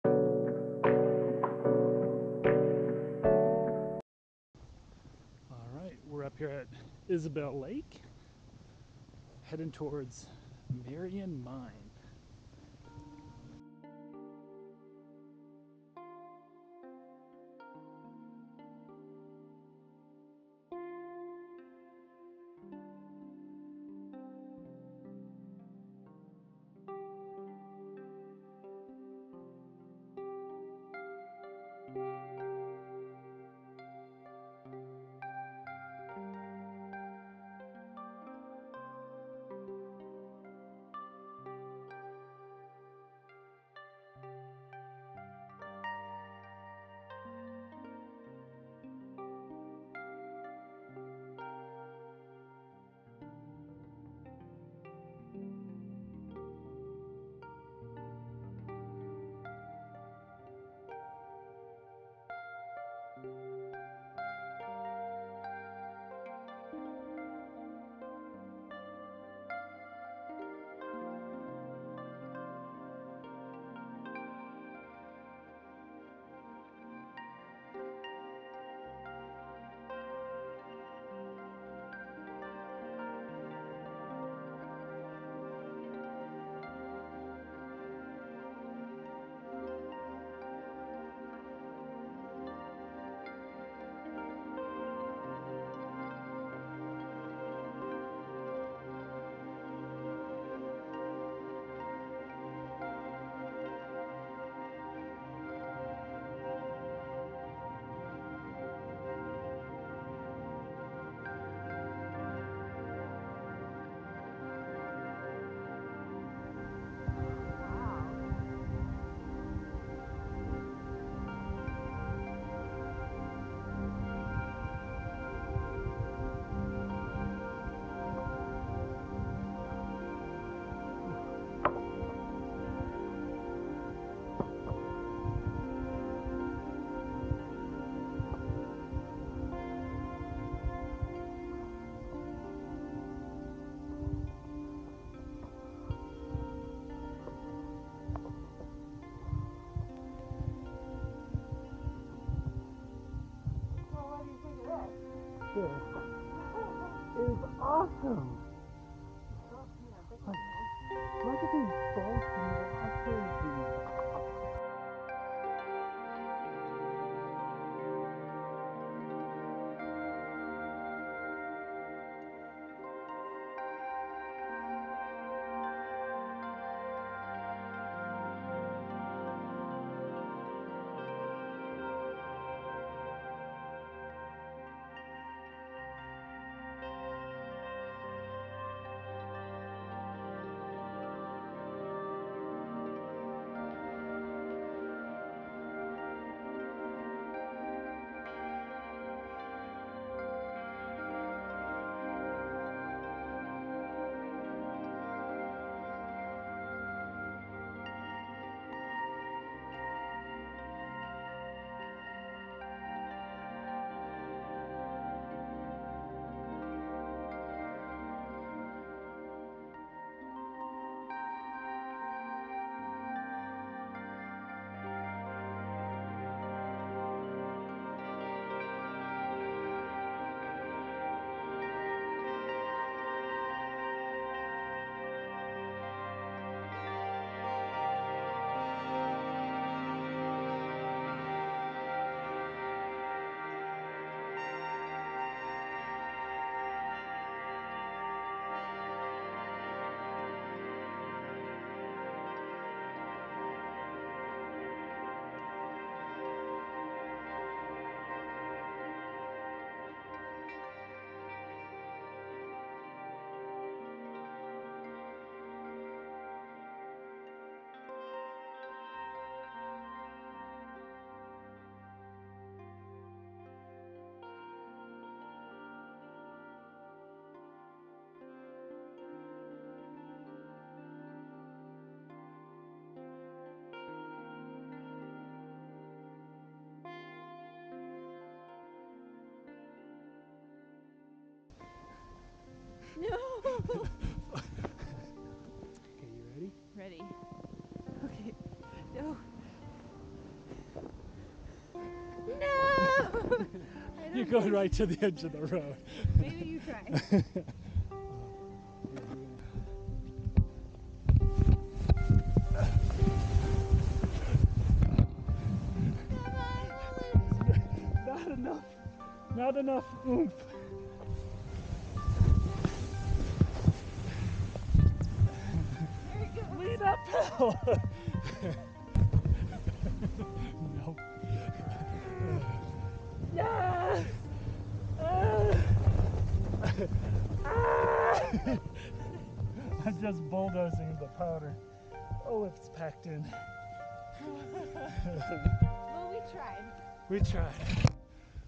Marion Mine Hike
Snowy day hike. 7 miles, a few inches of snow, and an amazing mine followed up with delicious snacks. Then we tried our hand at some sledding. Failed but when that sledding hill get's more snow. We will be there!